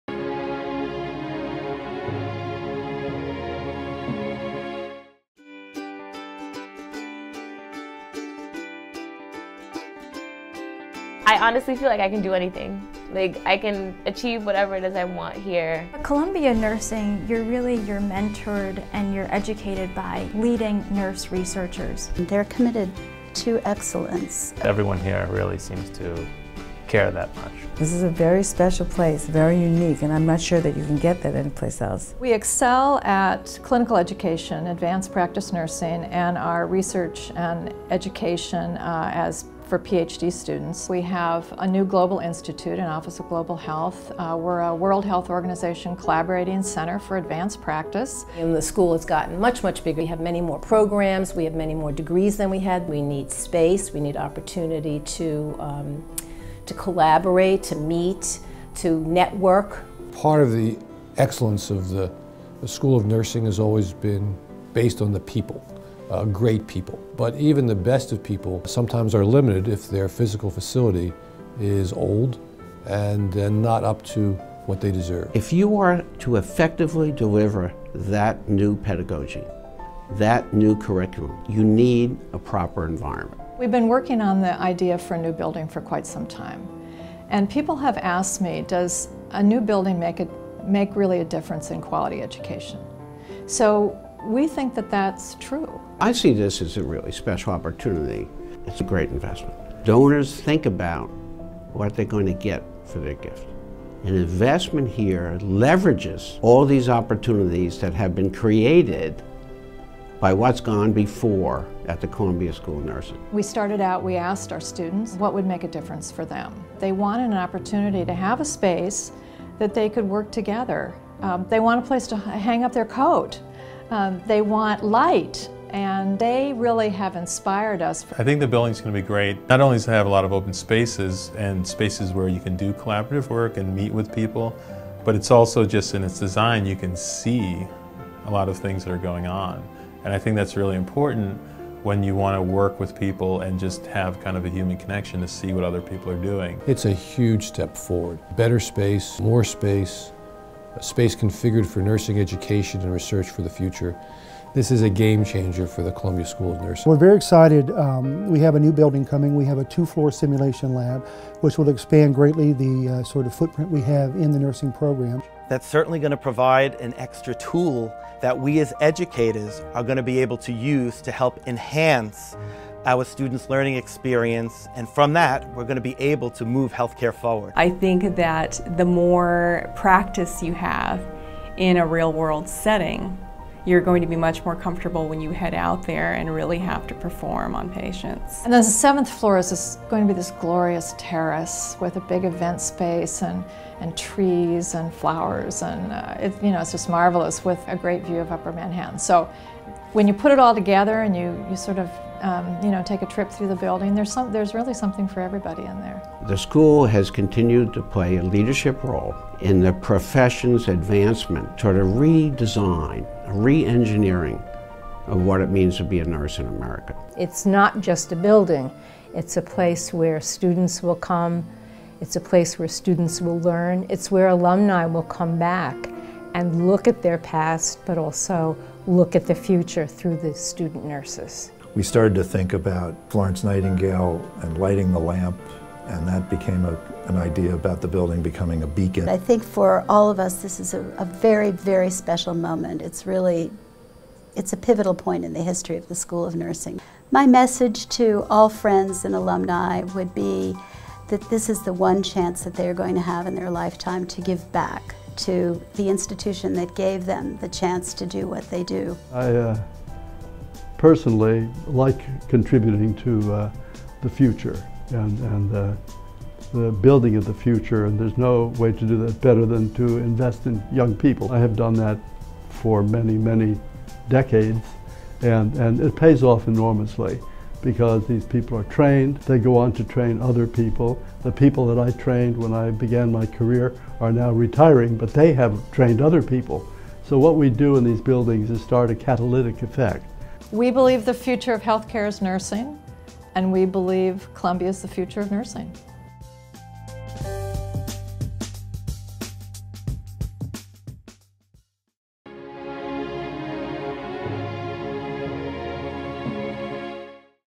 THE FUTURE OF NURSING IS COLUMBIA
Columbia University School of Nursing has an extraordinary opportunity as we prepare to build a new home for the future of nursing education, research, and clinical practice. Our new building will provide the space to ensure the highest standards of learning and research
that make Columbia Nursing one of the premier schools of nursing in the world.

Anchoring the east end of Columbia University Medical Center, the new building is configured to encourage collaboration, interaction, and clinical excellence. A state-
of-the-art simulation learning laboratory will prepare students for real-world complexities when caring for patients in a variety of clinical settings. Small and large meeting rooms will facilitate the exchange of information and ideas essential for innovative research. Faculty will
welcome students in their comfortable offices for one- on-one guidance and mentoring.

Our new home will feature a unified network of technologically advanced, flexible spaces that accommodate various styles of teaching and learning, as well as places for socializing and relaxation. For those occasions when students wish to work independently, the building will provide specially designed workstations for self-directed learning and quiet study.